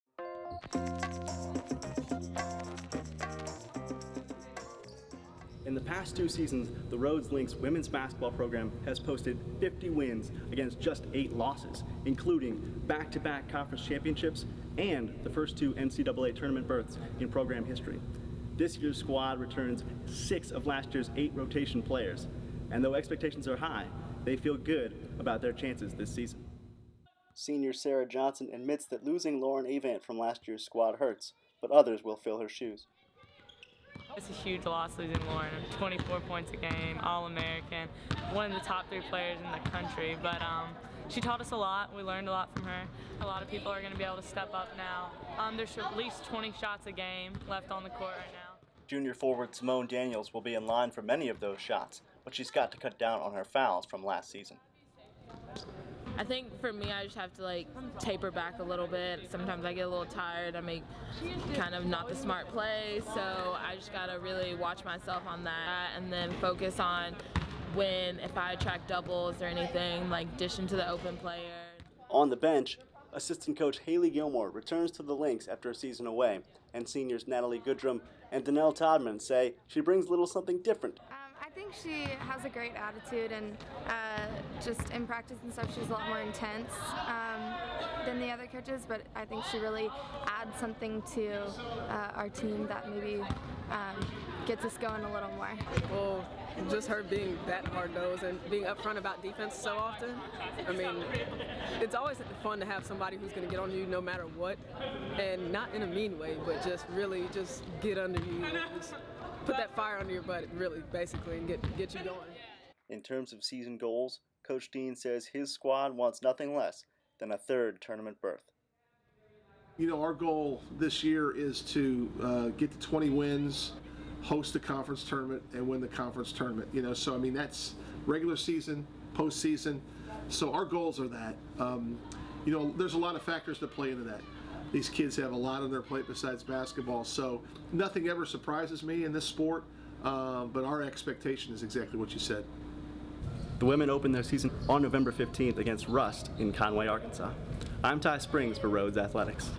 Rhodes College Women's Basketball 2014-2015 Season Preview
A look at the upcoming season for head coach Matt Dean and the Lynx hoops squad.